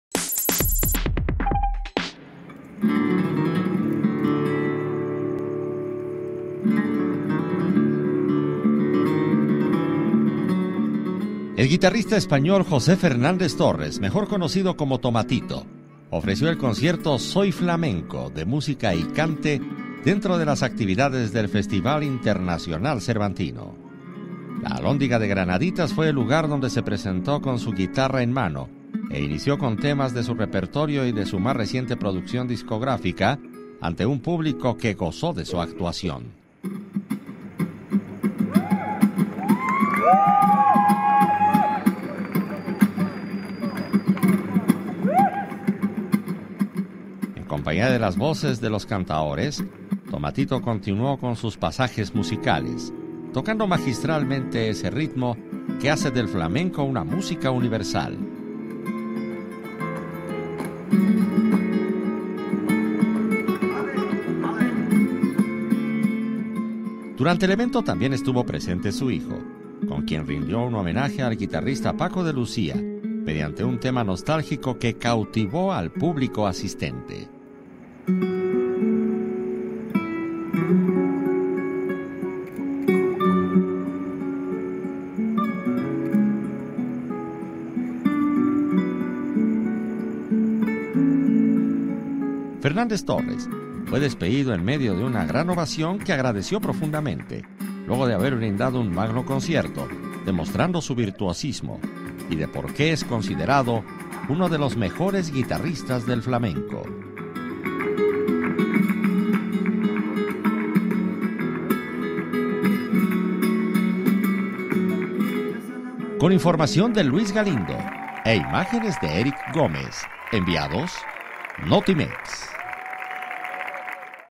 Ovación a “Tomatito” por su flamenco en el Festival Internacional Cervantino
La Alhóndiga de Granaditas fue el lugar donde el guitarrista español se presentó para ofrecer un concierto con temas de su repertorio y de su más reciente producción discográfica